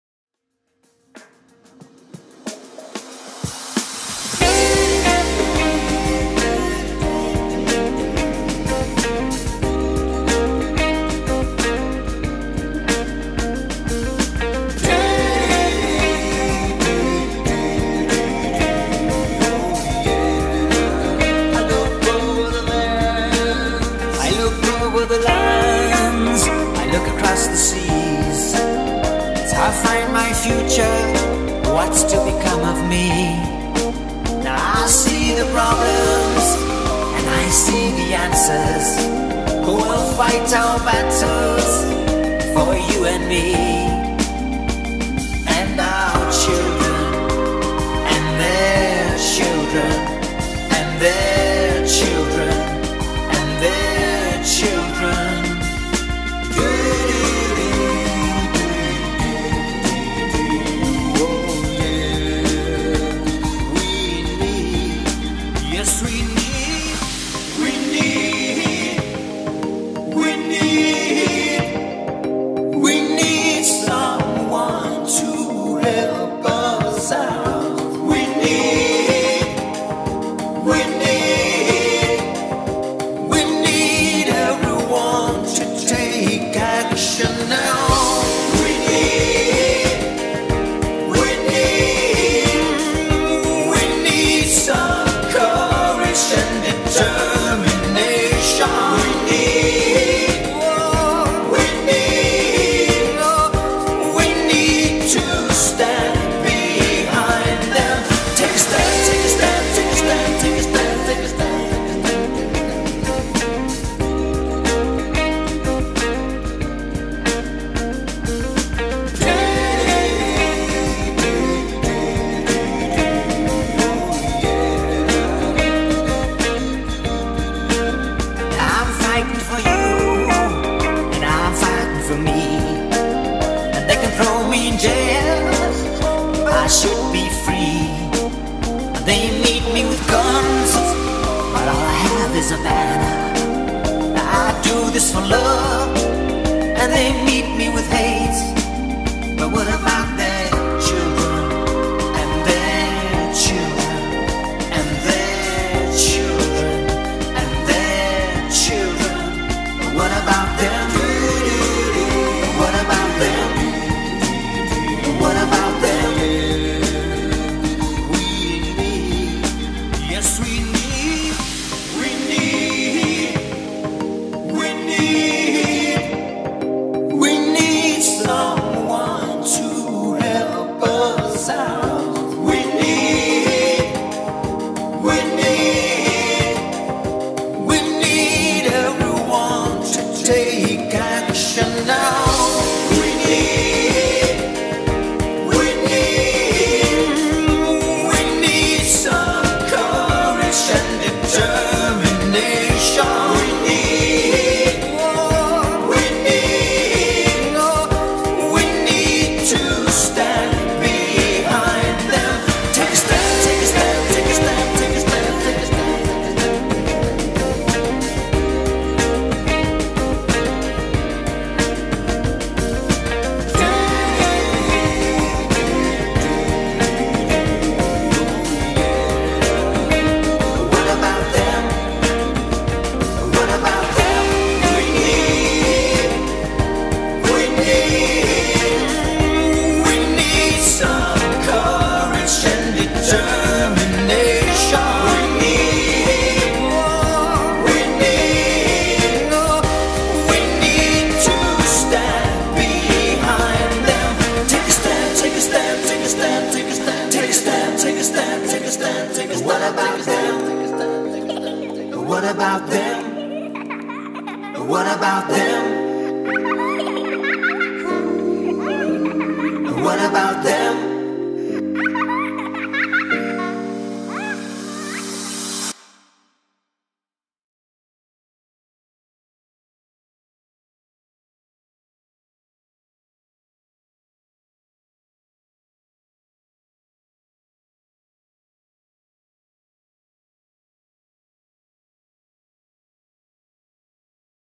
Greenpeace arctic tribute song.
This music-video, is a tribute to Greenpeace and The Arctic 30.

Lyrics: Jan Schnell.
Vocals and vocal melody: Jan Schnell.
Video mix: Jan Schnell

THIS VIDEO IS NOT FOR DOWNLOAD OR SALE!
Just for entertainment, fan, rehearsal and educational purposes.

Lyrics:

I look over the lands,I look across the sea
To find my future, what is to become of me
I see the problems, I see the answers
Who will fight the battles, for you and me
And our children…and their children…

We need….We need
We ne someone to help us out
We need….We need
Everyone to take action now
We need!…We need!
We need some courage and determination
We need!!…We need!!
To stand behind them
Take a stand, take a stand.

Now iI am fighting for you, and I am fighting for me
They can throw me in jail, but I should be free
They meet me with guns, and all I have is a banner
I do this for love, they meet me with hate
But what about their children…and their thildren

We need….We need
We need….We need
We need!…We need!
We need!!…We need!!